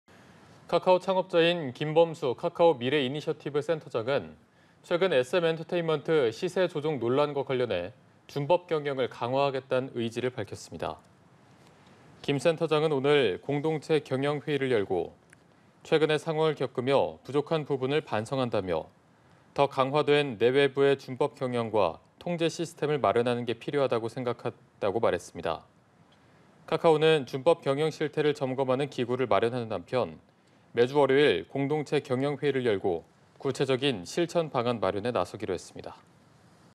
카카오 김범수 "나부터 반성…준법경영 시스템 마련" / 연합뉴스TV (YonhapnewsTV)
카카오 김범수 "나부터 반성…준법경영 시스템 마련"

카카오 창업자인 김범수 카카오 미래이니셔티브 센터장은 최근 SM엔터테인먼트 시세 조종 논란과 관련해 준법경영을 강화하겠다는 의지를 밝혔습니다.

김 센터장은 오늘(30일) 공동체 경영회의를 열어 "최근 상황을 겪으며 부족한 부분을 반성한다"며 "더 강화된 내외부의 준법 경영과 통제 시스템을 마련하는 게 필요하다고 생각했다"고 말했습니다.

카카오는 준법 경영 실태를 점검하는 기구를 마련하는 한편 매주 월요일 공동체 경영회의를 열고 구체적인 실천 방안 마련에 나서기로 했습니다.